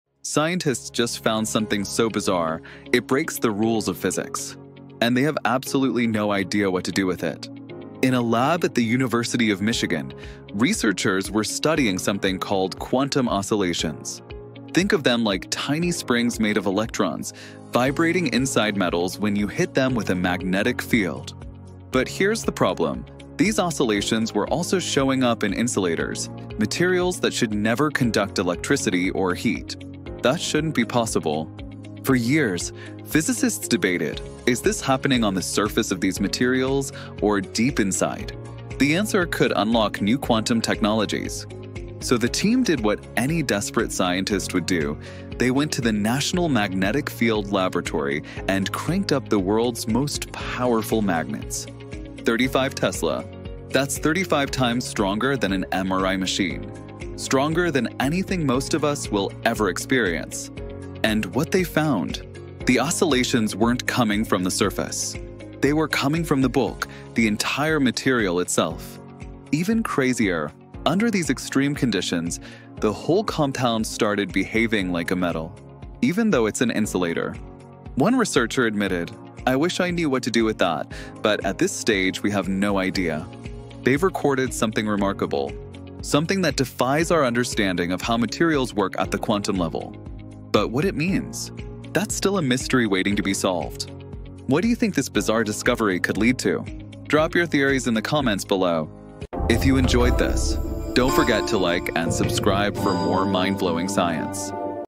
"Really Bizarre" Quantum Discovery Defies the Rules of Physics ⚛️
University of Michigan researchers discovered something impossible: quantum oscillations in an insulator material that behaves like a metal under extreme magnetic fields. Using the world's most powerful magnets (35 Tesla), they found the effect comes from deep inside the material—not the surface. The catch? Scientists have no idea what it means yet. 🧲